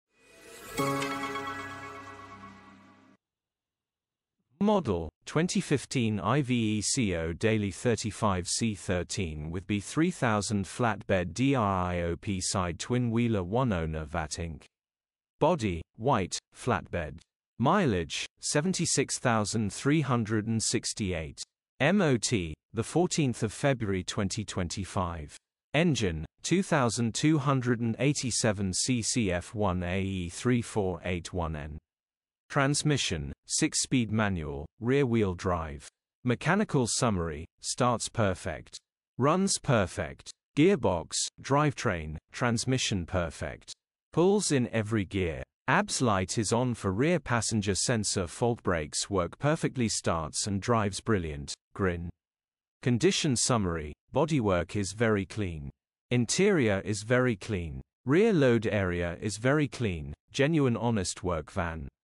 NV64EWG - 2015 Iveco Daily 35C13 flat bed drop side twin wheeler 6 speed manual 76k miles vat incl
Model: 2015 IVECO DAILY 35C13 W/B 3000 FLAT BED DRIOP SIDE TWIN WHEELER 1 OWNER VAT INC
Body: White - Flatbed
Mileage: 76368
MOT: 14th February 2025
Engine: 2287cc F1AE3481-N
Transmission: 6 Speed Manual - RWD
Mechanical Summary: 🟢 Starts perfect. Runs perfect. Gearbox, drivetrain, transmission perfect. Pulls in every gear. ABS light is on for rear passenger sensor fault brakes work perfectly starts and drives brilliant.😀
Features: 0 Former Keepers, 2 Keys, Electric Windows
EU Status: Euro 5
HPI: Clear